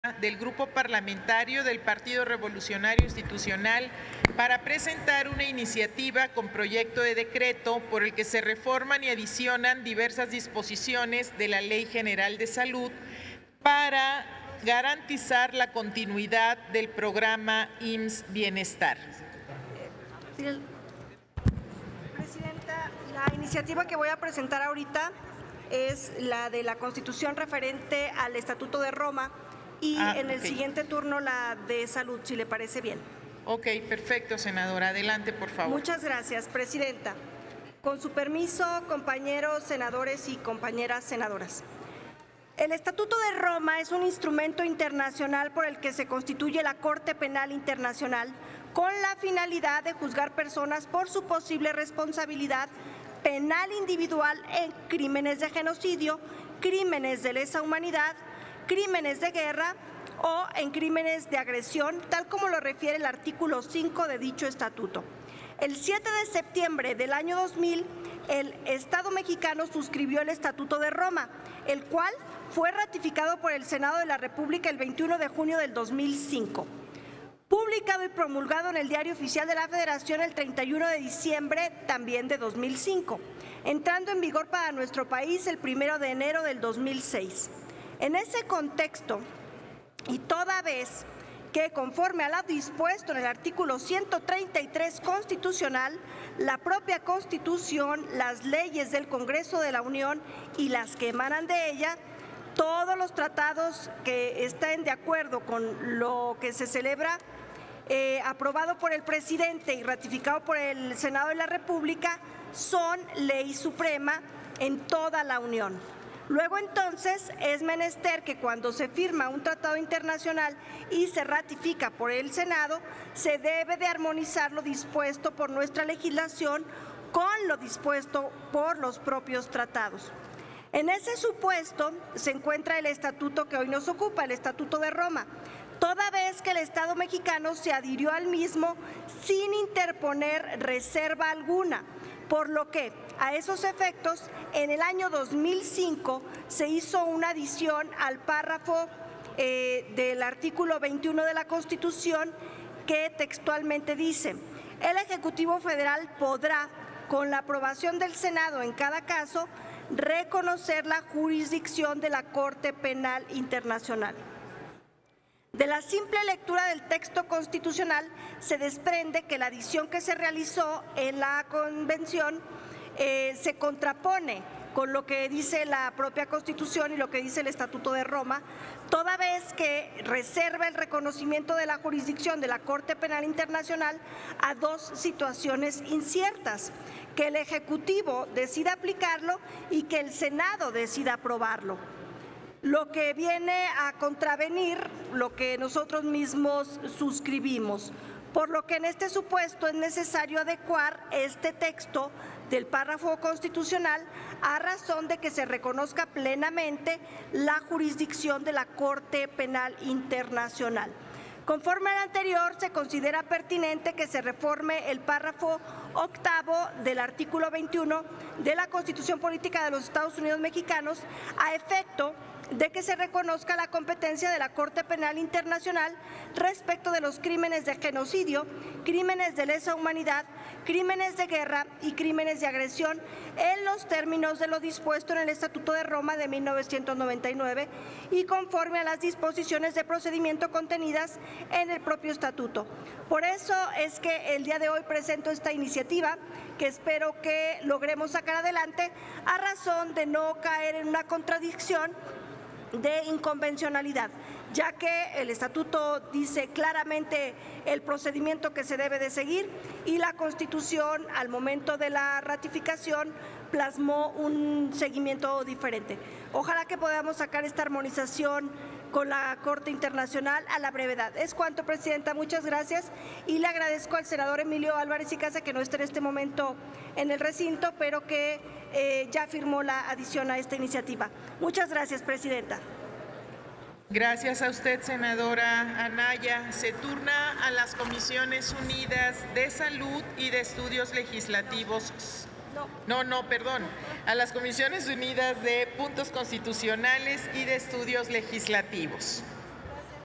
Reconocer plenamente jurisdicción de Corte Penal Internacional, busca Sen. Claudia Anaya (PRI)
La senadora Claudia Anaya, del PRI, señala que dicha instancia tiene jurisdicción en materia de crímenes de genocidio, lesa humanidad, de guerra o agresión. Apunta a los instrumentos internacionales firmados por México y a la necesidad de armonizar las leyes nacionales .

Lo anterior, al presentar una iniciativa para reformar el artículo 21 de la Constitución.